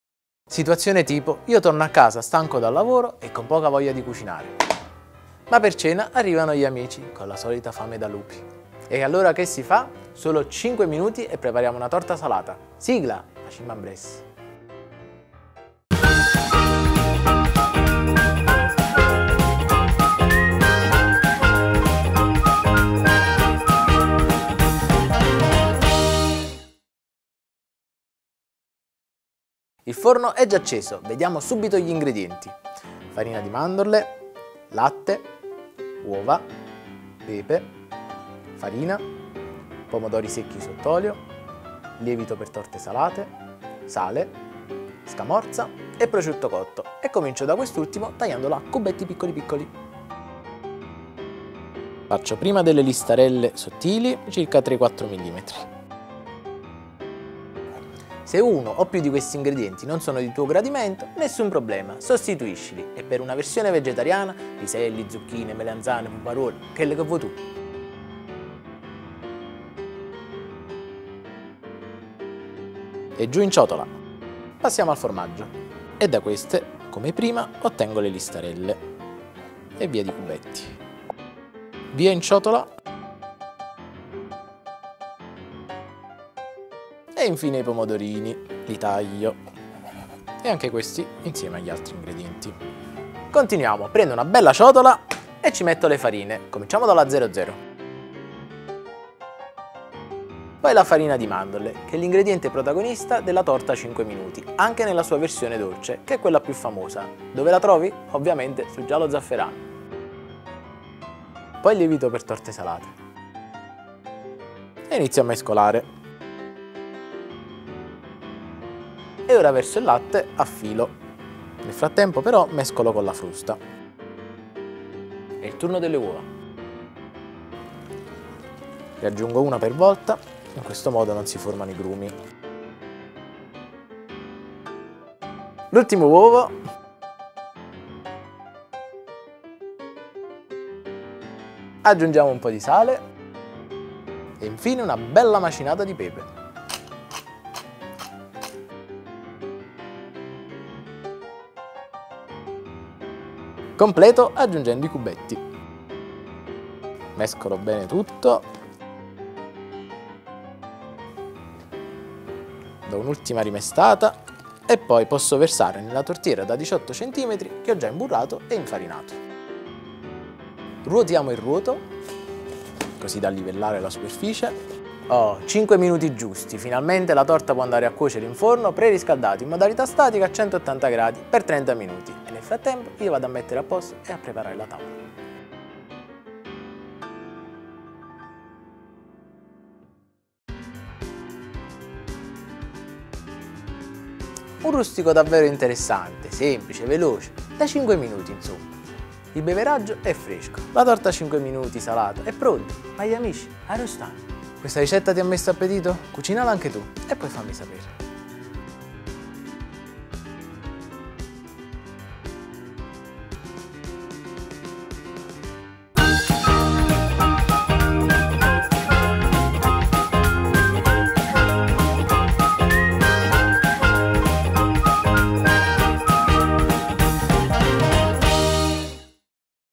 Torta 5 minuti salata
La torta 5 minuti salata è un'idea sfiziosa e veloce da servire come antipasto, perfetta quando si hanno ospiti improvvisi! GUARDA ANCHE LA FOTORICETTA